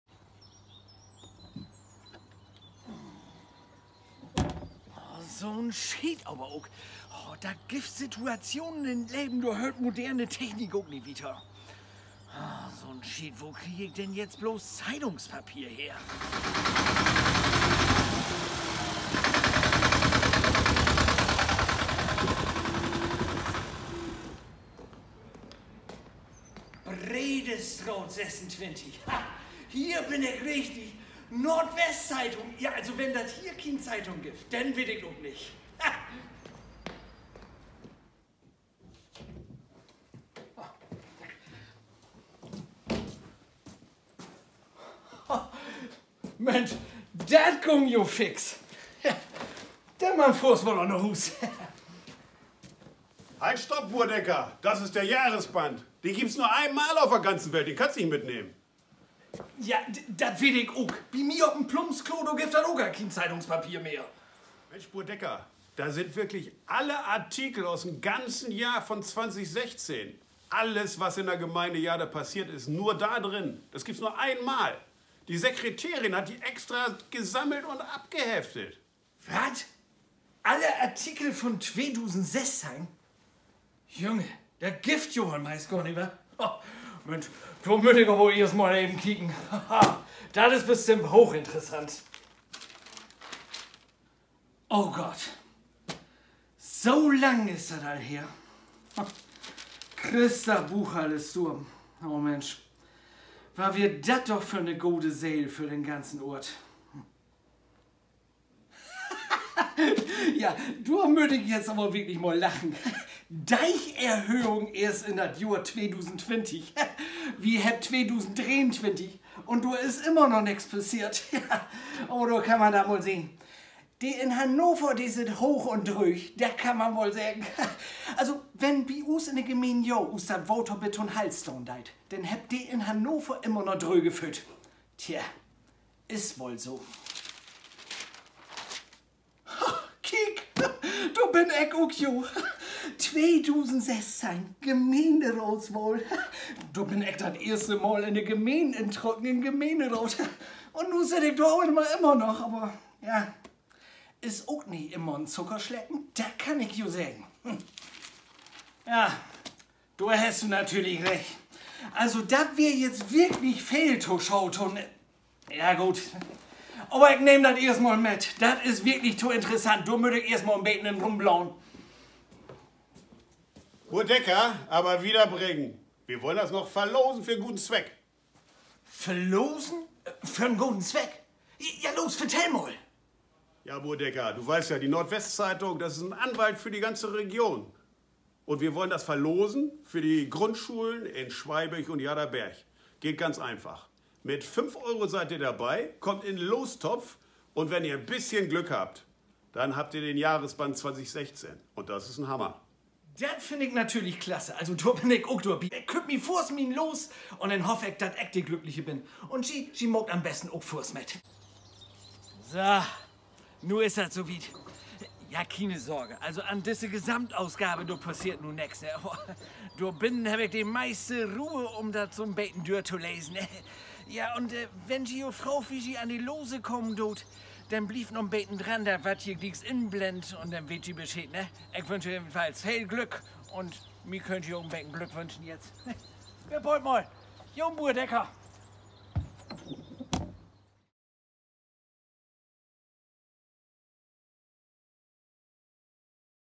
Buur Decker und die NWZ
Tolle Aktion! Die NWZ und Buur Decker verlosen den Jahresband 2016 der NWZ. Alle Artikeln aus der Gemeinde Jade aus dem Jahr auf originalem Zeitungspapier! Eine Rarität, die im digitalen Zeitalter sicher auch im Wert steigt.